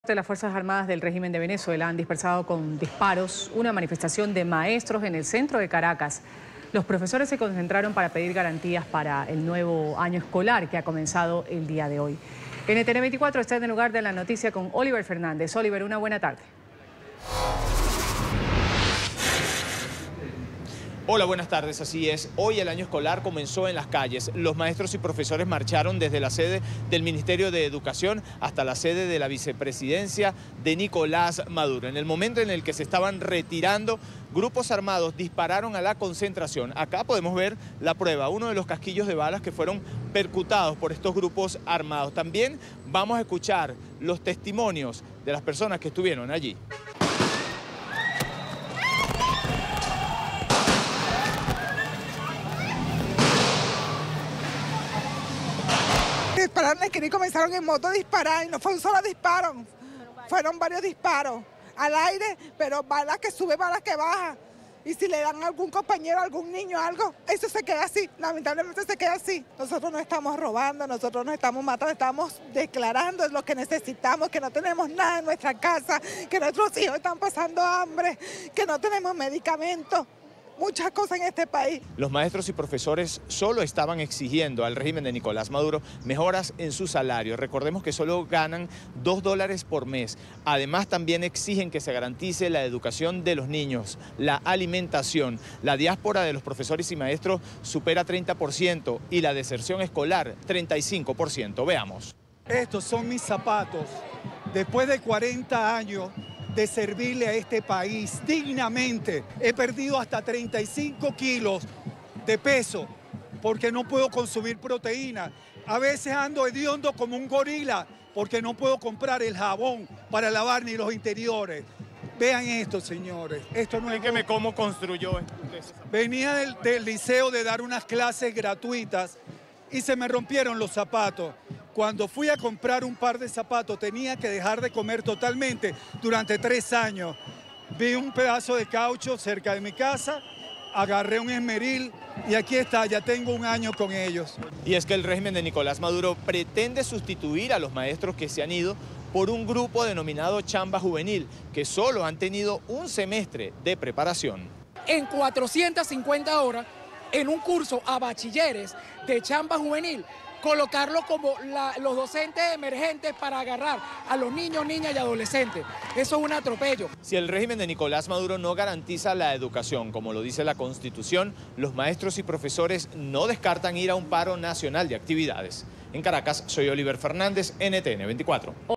Régimen de Maduro dispersó con disparos al aire a profesores que manifestaban en Caracas
Las Fuerzas Armadas del régimen venezolano dispersaron con disparos a una manifestación de maestros que se estaba llevando a cabo en el centro de Caracas. Los profesores se concentraron para pedir garantías para el nuevo año escolar que comenzó este lunes.